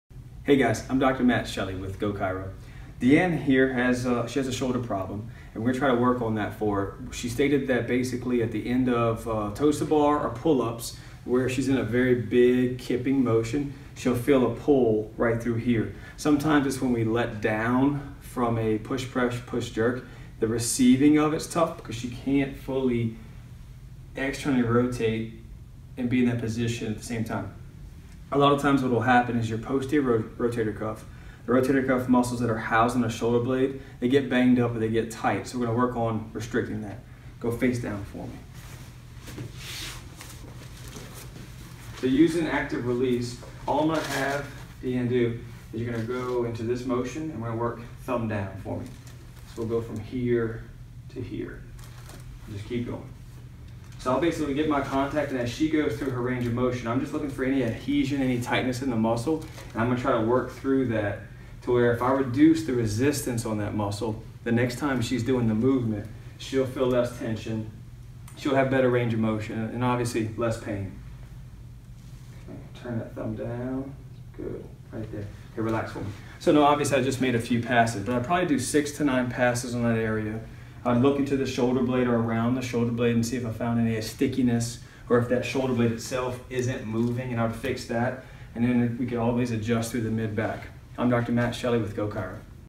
Geaux Chiro Hammond | Shoulder Pain
Hey guys, I'm Dr. Matt Shelly with Geaux Chiro. D'ann here has a, she has a shoulder problem and we're trying to work on that for her. She stated that basically at the end of a toes to bar or pull ups where she's in a very big kipping motion, she'll feel a pull right through here. Sometimes it's when we let down from a push press, push jerk, the receiving of it is tough cause she can't fully externally rotate and be in that position at the same time. A lot of times what will happen is your posterior rotator cuff, the rotator cuff muscles that are housed in the shoulder blade, they get banged up or they get tight. So we're gonna work on restricting that go face down.

They use an active release, all I'm going to have D'ann do is you're going to go into this motion and to work thumbed down for me. So we'll go from here to here. Just keep going. So I'll basically to get my contact and that she goes through her range of motion. I'm just looking for any adhesion, any tightness in the muscle, and I'm going to try to work through that to where if I were to do so, the resistance on that muscle, the next time she's doing the movement, she'll feel less tension. She'll have better range of motion and obviously less pain.

Turn that thumb down. Good, right there.

Okay relax for me. So no, obviously I just made a few passes, but I'd probably do six to nine passes in that area and looking to the shoulder blade around the shoulder blade and see if I found any stickiness or if that shoulder blade itself isn't moving and how to fix that. And then we can always adjust to the mid back. I'm Dr Matt Shelly with Geaux Chiro.